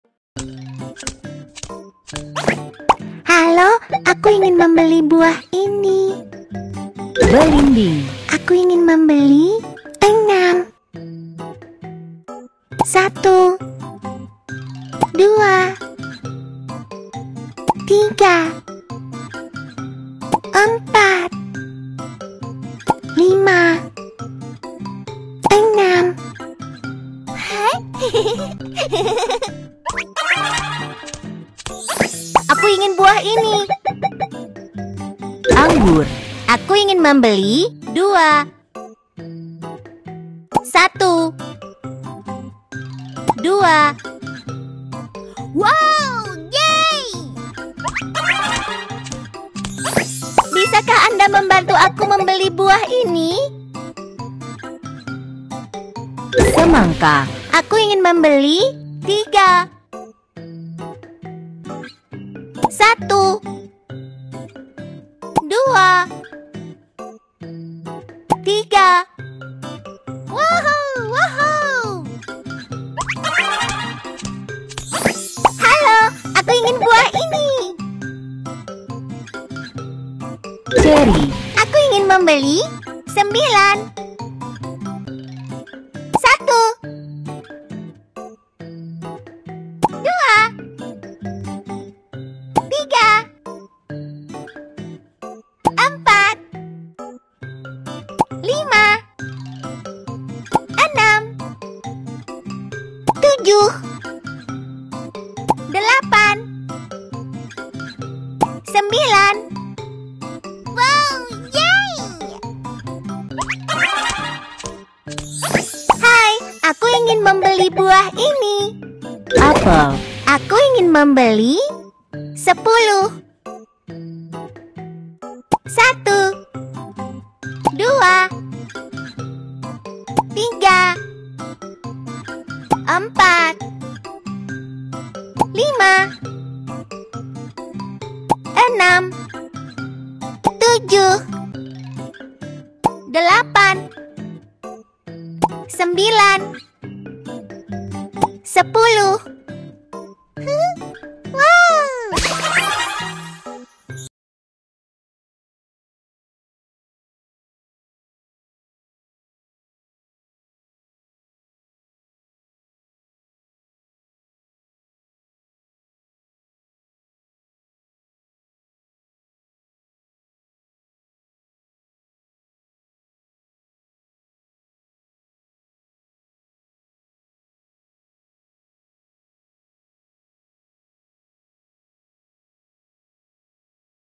Berhitung Dengan Media Buah, Game Baby Bus
Anak anak belajar berhitung dengan media buah